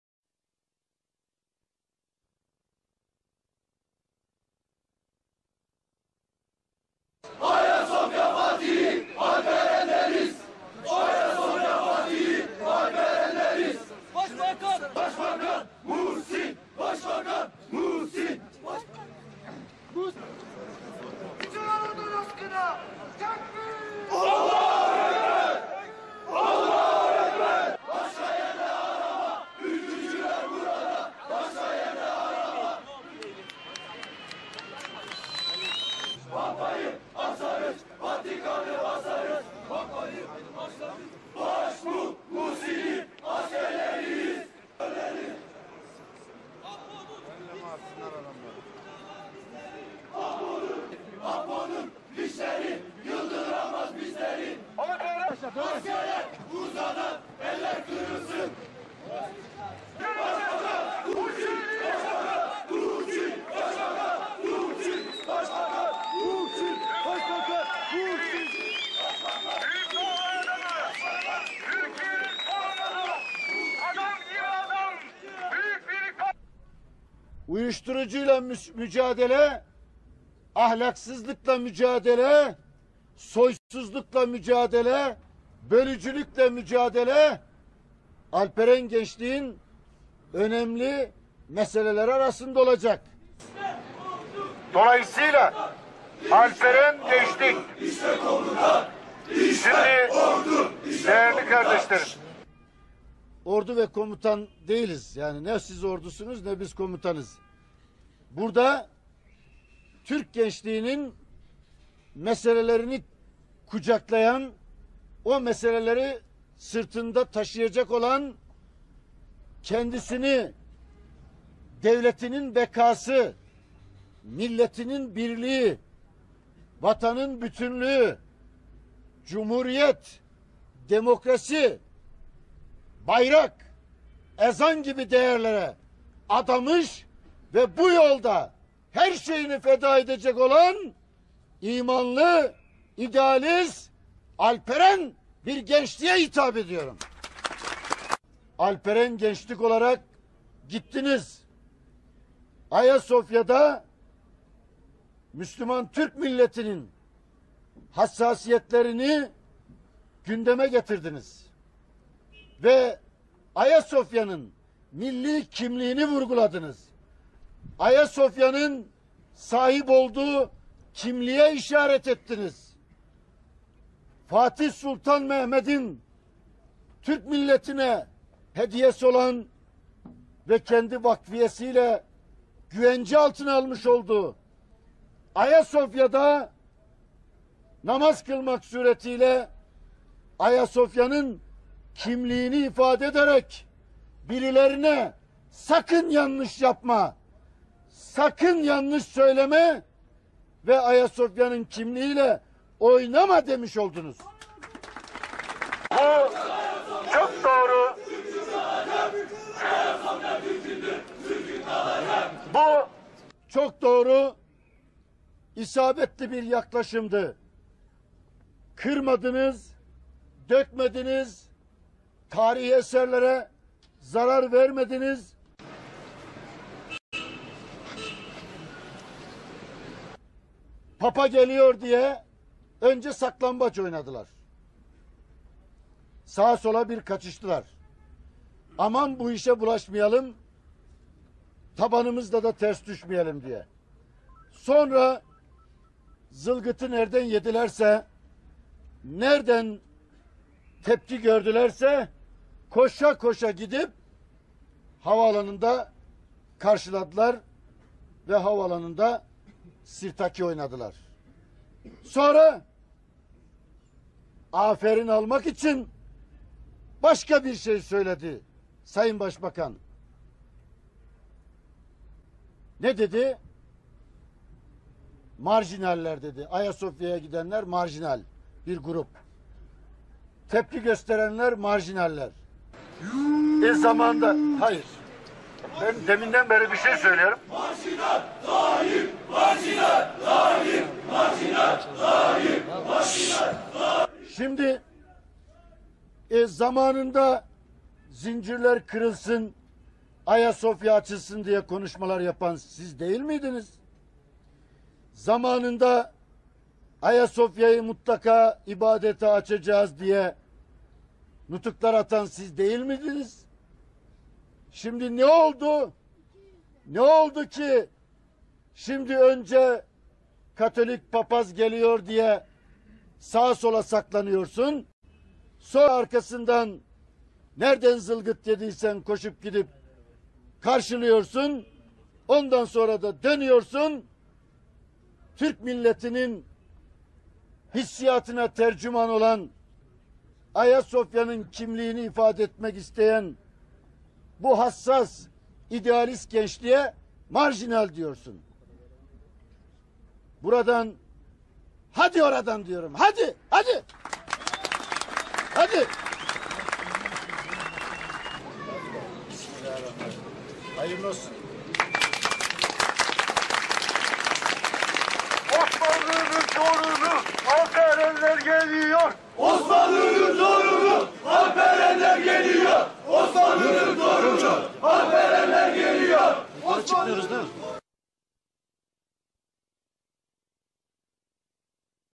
Muhsin Yazıcıoğlu | Erdoğan - Papa Görüşmesini Eleştirdi
"Papa geliyor diye önce saklambaç oynadılar, sağa sola kaçıştılar -aman bu işe bulaşmayalım, tabanımız ile ters düşmeyelim- diye. Sonra zılgıtı nereden yedilerse, nereden tepki gördülerse koşa koşa gittiler havaalanında karşıladılar ve havaalanında sirtaki oynadılar."

BBP Genel Başkanı Muhsin Yazıcıoğlu, Alperen Ocağı Genel Merkezi'nin açılışını yaptı. Burada bir konuşma yapan Yazıcıoğlu, Türk gençliğinin yabancı kültür ve fikirlerin saldırısı ile karşı karşıya olduğunu vurguladı. Uyuşturucunun ortaokula kadar indiğini belirten Yazıcıoğlu, "Sokaklar kapkaçtan, gasptan yürünemez duruma geldi. Gençliğimizde büyük bir ahlaki çöküntü, dejenerasyon görmekteyiz. Türk gençliğini uyuşturucunun, fuhuşun, ahlaki yozlaşmanın, terörün, şiddetin kollarından alacak; onları Türk tarihinin sevgi medeniyeti ile oluşturduğu kültür ve değerlerimizle donatacağız. Kin, nefret, kavga, şiddet girdabından Türk gençliğini çıkartıp; sevgi medeniyetinin şevkatli kollarına atacağız." diye konuştu.

Alperen Ocağının, Türkiye'nin her köşesinde gençliğin sorunlarına çözüm üretmeye çalışacağını kaydeden Yazıcıoğlu, " Uyuşturucu ile mücadele, ahlaksızlıkla mücadale, bölücülükle mücadele Alperen gençliğin önemli meseleleri arasında olacak." ifadesini kullandı.

"NE SİZ ORDUSUNUZ NE BEN KOMUTAN"

Konuşması sırasında atılan "İşte ordu, işte komutan" sloganlarını, "Ne siz ordusunuz, ne ben komutan" sözleriyle engelleyen Yazıcıoğlu, "Burada Türk gençliğinin meselelerini kucaklayan, o meseleleri sırtında taşıyacak olan kendisini devletinin bekası, milletinin birliği, vatanın bütünlüğü, cumhuriyet, demokrasi, bayrak, ezan gibi değerlere adamış ve bu yolda her şeyini feda etmeye hazır alperen bir gençliğe hitap ediyorum" dedi.

Yazıcıoğlu kendisini dinleyen gençlere, "Şiddetten yana olmayacaksınız. Silahınız fikirleriniz olacak. Asla şiddete yer yok. Asla şiddetle, terörle fikirlerimizi kabul ettirmeye çalışmayacağız. Savunmamız da mücadelemiz de fikirlerimizle olacak." diye seslendi.

Papa 16. Benedict'in Türkiye ziyareti öncesinde Alperen Ocağı'ndan gençlerin Ayasofya'da kıldığı namazı hatırlatan Yazıcıoğlu, "Ayasofya'da Müslüman Türk milletinin hassasiyetlerini gündeme getirdiniz, Ayasofya'nın milli kimliğini vurguladınız" şeklinde konuştu.

Başbakan Recep Tayyip Erdoğan'ın Papa 16. Benedict ile havaalanında yaptığı görüşmeyi de eleştiren Yazıcıoğlu şöyle devam etti: "Papa geliyor diye önce saklambaç oynadılar, sağa sola kaçıştılar -aman bu işe bulaşmayalım, tabanımız ile ters düşmeyelim- diye. Sonra zılgıtı nereden yedilerse, nereden tepki gördülerse koşa koşa gittiler havaalanında karşıladılar ve havaalanında sirtaki oynadılar."

Yazıcoğlu'nun Ayasofya'daki eylem için Erdoğan'ın marjinal ifadesini kullandığını hatırlatması üzerine, yuhalamalar yükseldi. Yazıcıoğlu'nun yuhalamaları engellemesi üzerine grup, "Marjinal Tayyip" sloganları attı.

Konuşmasına devam eden Yazıcıoğlu, "Zamanında zincirler kırılsın, Ayasofya açılsın diye konuşmalar yapan siz değil miydiniz? Zamanında Ayasofya'yı ibadete açacağız diyen siz değil miydiniz?" diye sordu.

Yazıcoğlu şunları dile getirdi: "Ne olduki, önce Katalik Papaz geliyor diye sağ sola saklanıyorsun, arkasından nereden zılgıt yediysen koşup gidip karşılıyorsun; sonra da dönüyorsun Türk milletinin hissiyatına tercüme olan Ayasofya'nın kimliğini ifade etmek isteyen bu hassas idealist gençliğe marjinal diyorsun. Buradan - hadi oradan - diyorum; -hadi hadi-. Asıl marjinal sizsiniz. Muhalefetinizde, iktidarınızda her dönemde farklı farklı konuşan ve davranan sizlersiniz. Rüzgar gülü gibisiniz. Rüzgar ne taraftan esiyorsa o tarafa gidiyorsunuz. Salkım söğütler gibisiniz. Hep aşağıya doğru eğiliyorsunuz"